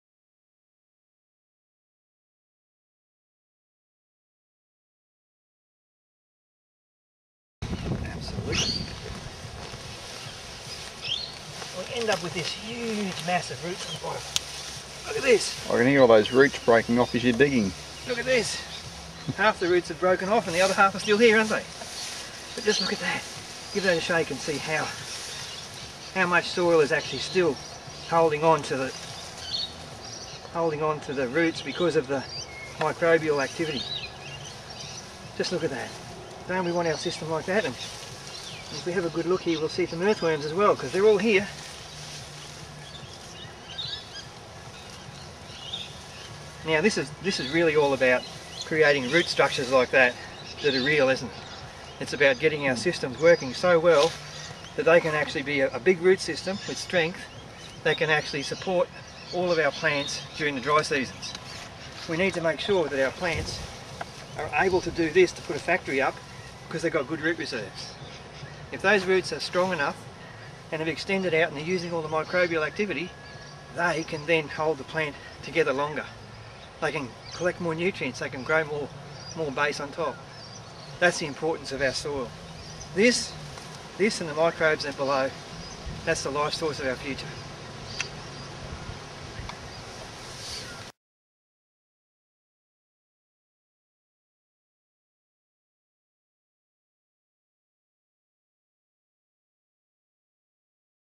Technology of Growing Grass
Mick Alexander of Grazing BestPrac in Queensland Australia is describing the most important lining processes on earth. The combination of photosynthesis, carbon/ energy transfer, microbial activity and nutrient transfer into the living plants. Mick runs a series of workshops and support services for graziers and farmers in Australia with the goal of improving production, health , profit and lifestyle. It all starts with the soil.